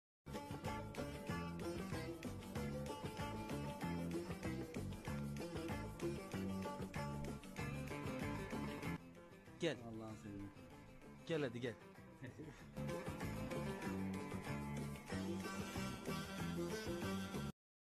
Kocaeli'de kahvehane baskininda ceza yememek için oturup üzerini örterek saklanan kişi yakaladı
Kocaeli'de bir kahvehaneye baskın düzenleyen polis, ceza yememek için yere oturan ve üzerine çarşaf örterek saklanmaya çalışan kişiyi ayaklarının açık kalması sonucu yakaladı.